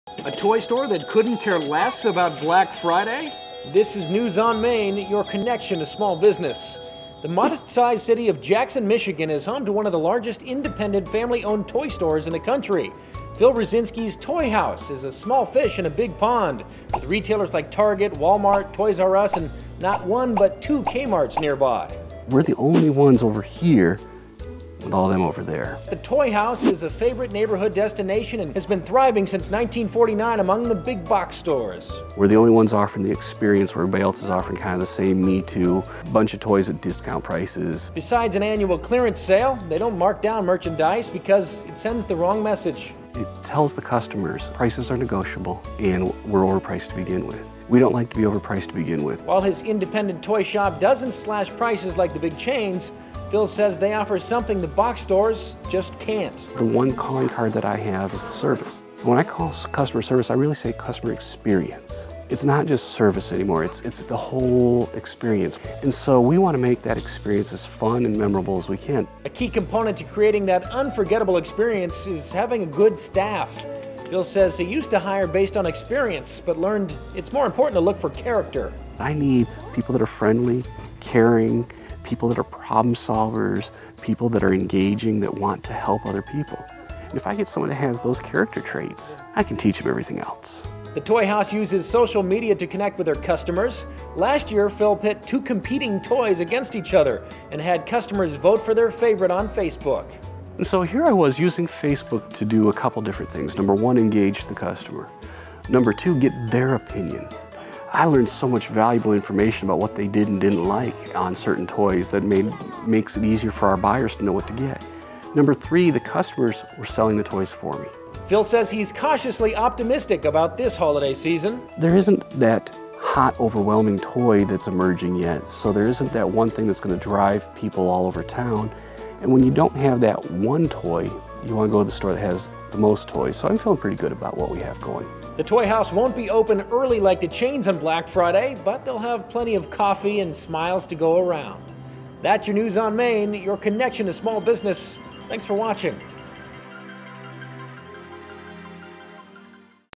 A Neighborhood Toy Store Takes on the Big Box Retailers
News on Main (11/21/11): An independent toy-store owner, who shuns deep discounts on Black Friday, shares his secrets to success against the likes of Target and Wal-Mart.